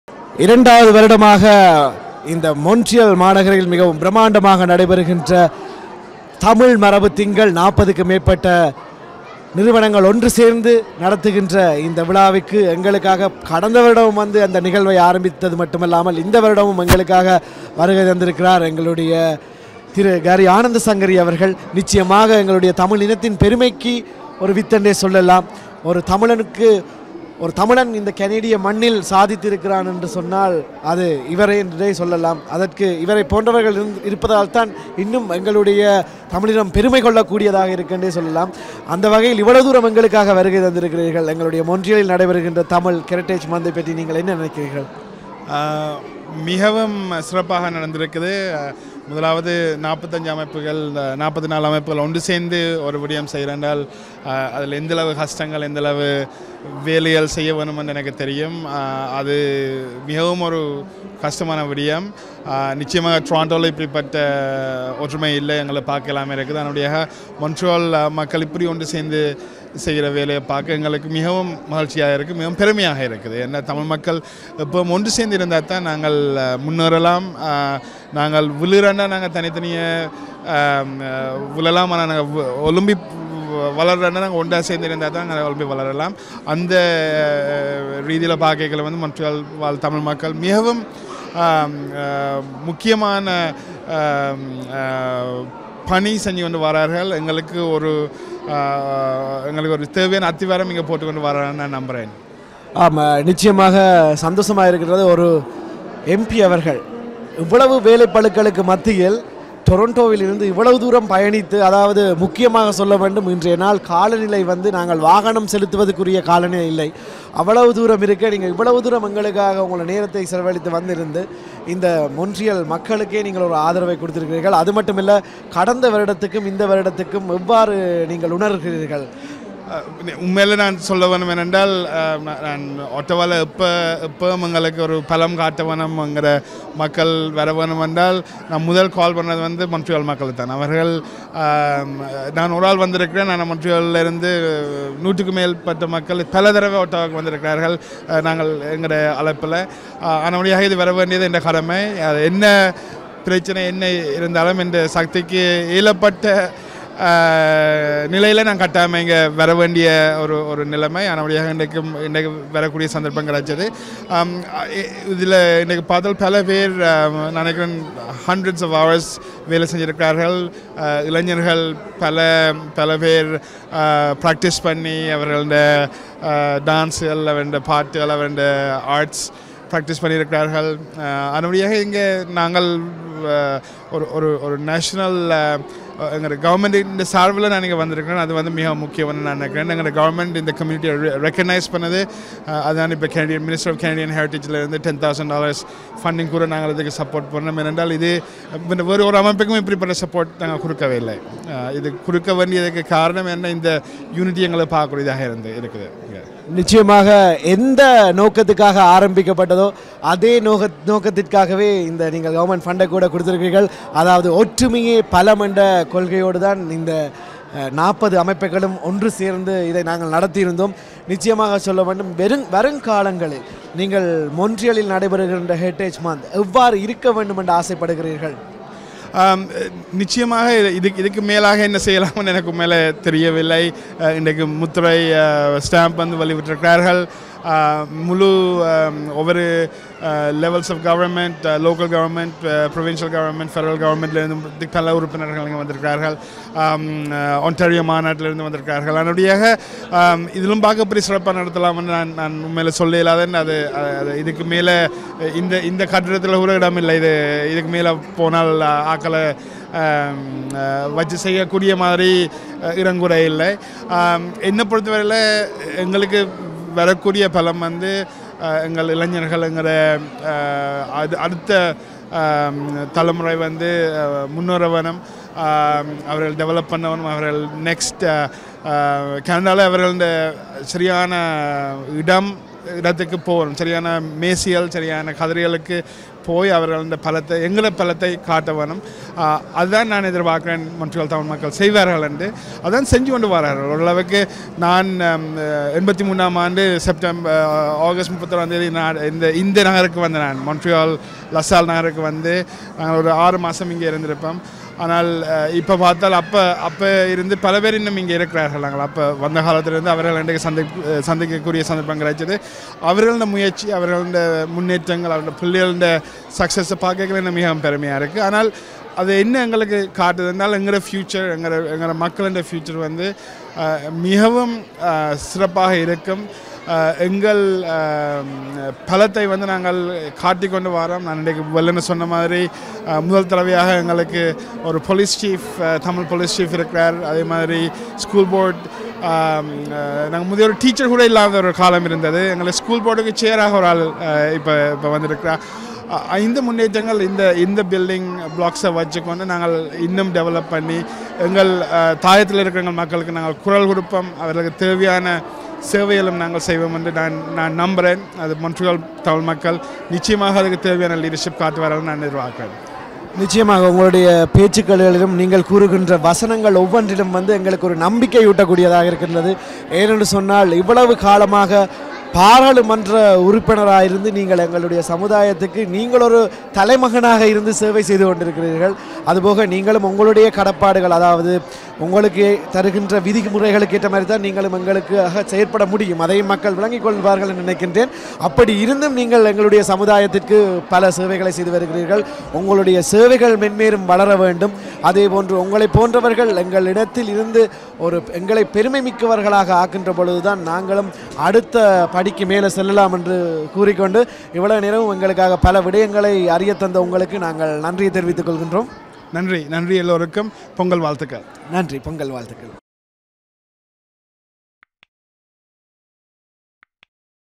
Gary Anandasangaree on Quebec Tamil Heritage Month 2020 Celebrations
[Tamil Heritage Month 2020] Interview with Gary Anandasangaree